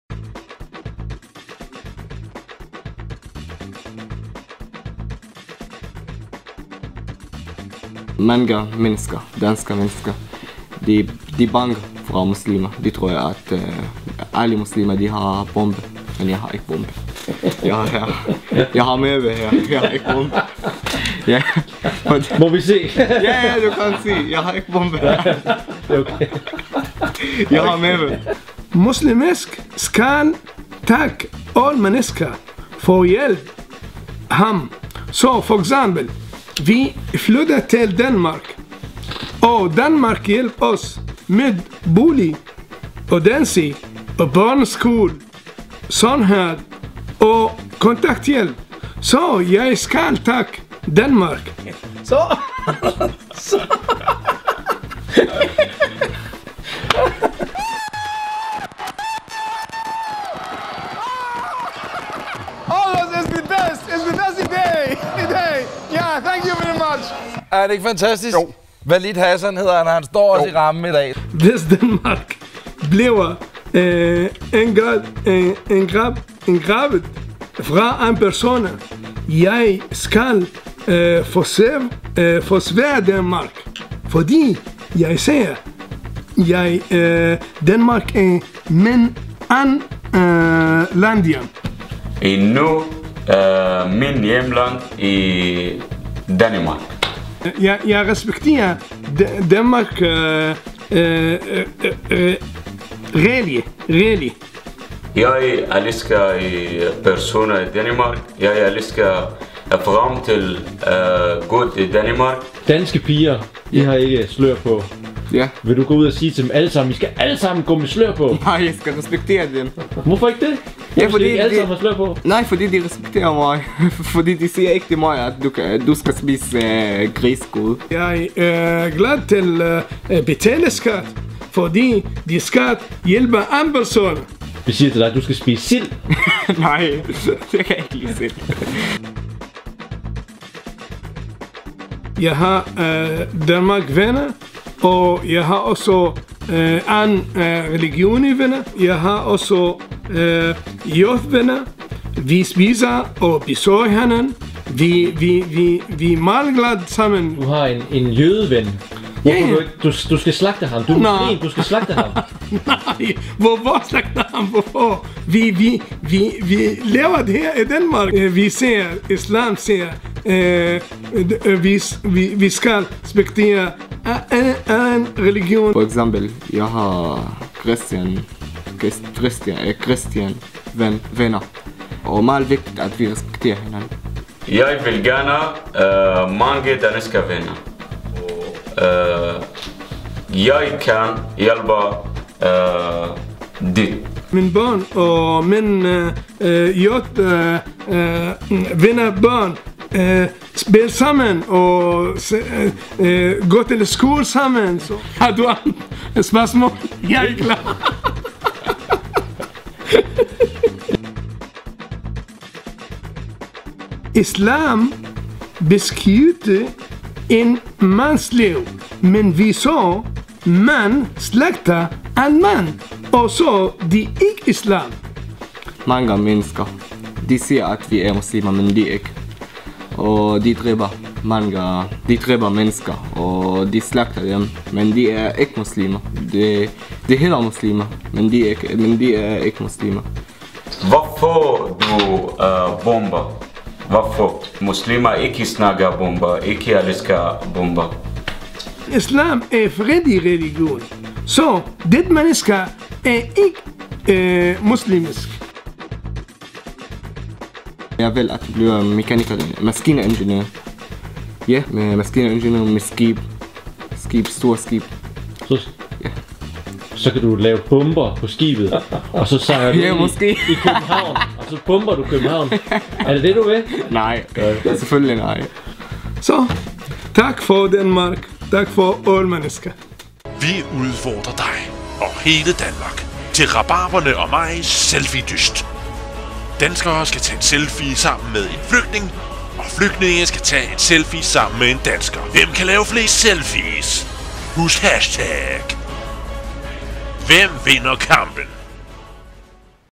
Rabarberne & mig #8 ISLAM OG DANMARK
Efter terrorangrebene i Belgien og Pakistan, samt TV2's "Moskeerne bag sløret" så har Rabarberne noget de gerne vil sige til jer alle.
Denne video vil glæde de fleste danskere